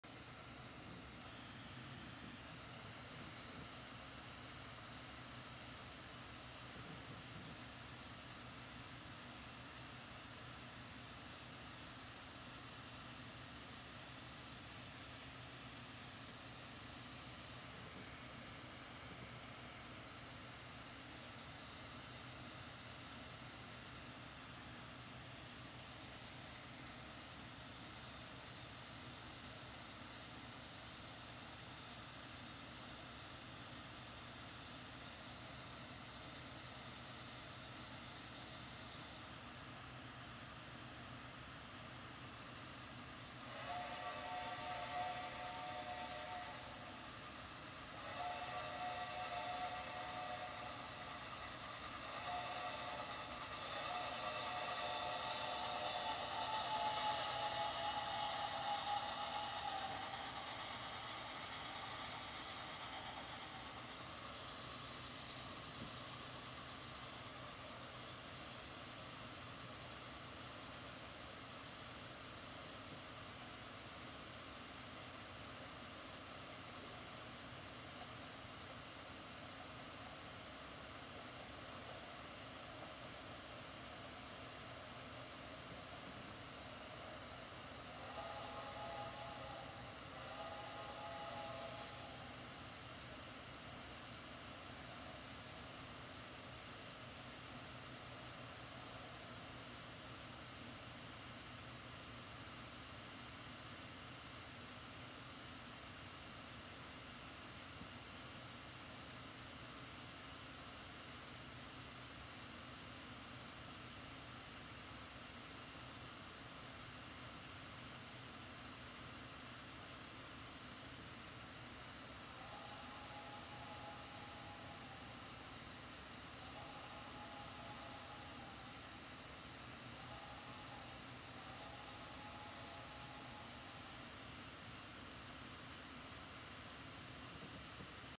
Reading and Northern 2102 and 425 pulling a 23 car passenger train fall foliage excursion!
Reading and Northern 4-8-4 #2102 and 4-6-2 #425 doubleheader a 23 car passenger train fall foliage excursion!